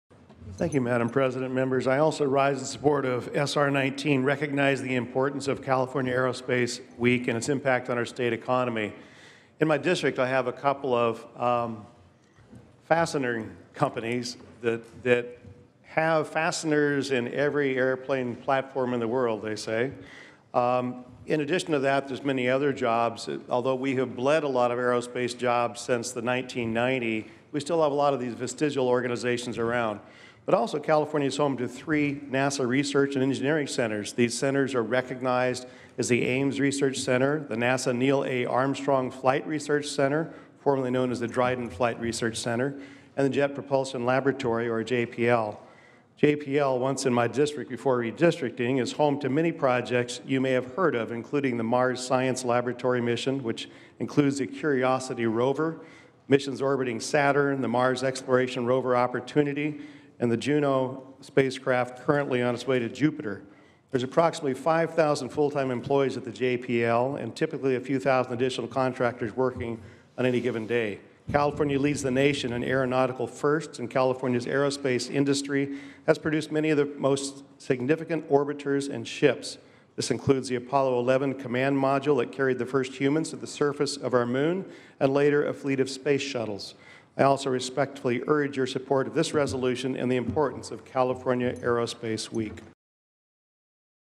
California Aerospace Week
Senate Minority Leader Bob Huff (R-Diamond Bar) rises to speak in support of a Senate Resolution honoring the role of the aeronautical industry in California and California Aerospace Week. California has been at the forefront of aeronautical development and is home to businesses responsible for crafting the Apollo landing craft that placed the first men on the moon, and later, a fleet of space shuttles. Senator Huff serves as the Senate Minority Leader and represents the 29th Senate District covering portions of Los Angeles, Orange and San Bernardino Counties. Follow Senator Huff on Twitter @bobhuff99.